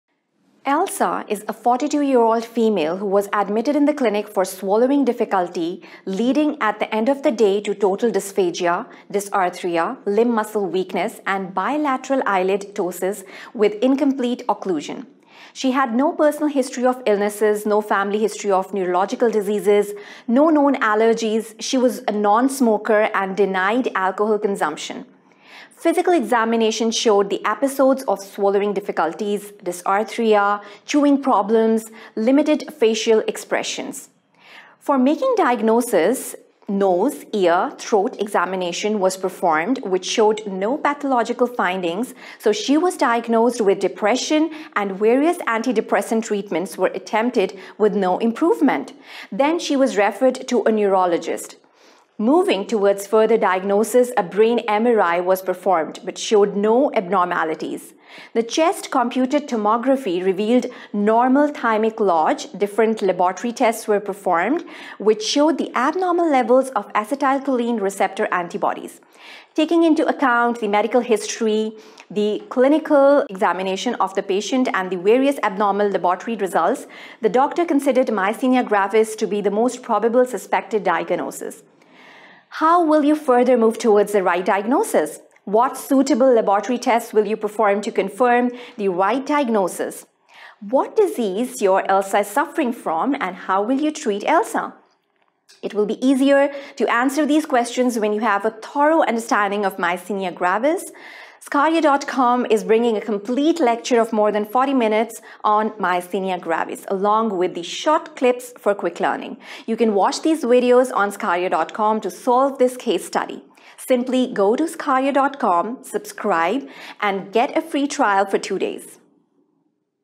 Myasthenia Gravis | Neurology Medicine Case Study | Medical Education | V-Learning
We came across a female patient. Her name is Elsa. In the case study of Elsa, we noted that she was admitted to the clinic for swallowing difficulty and how after some time she developed dysphagia, dysarthria, limb muscle weakness, and bilateral eyelids ptosis with incomplete occlusion. These signs took the physician´s concern and after the physical examination, a suspected diagnosis is set down. On the basis of suspected diagnosis, we at sqadia.com has made a lecture on Myasthenia Gravis for your understanding.

This Neurology Medicine Myasthenia Gravis lecture discusses its pathophysiology, clinical features, and differential diagnosis. Along with this, several diseases associated with it comes under consideration. In the end, this V-Learning™ explains the treatment.

Lecture Duration - 00:50:32

Myasthenia gravis is one of the Neuromuscular Disorders primarily characterized by Muscles weakness and Muscles fatigue. Myasthenia gravis neurology student lecture begins by explaining its pathophysiology. For your ease, the pathophysiology of myasthenia gravis is made understandable by comparing the diagrams of the normal neuron and the one undergone MG.

1400+ Medical Courses Lectures.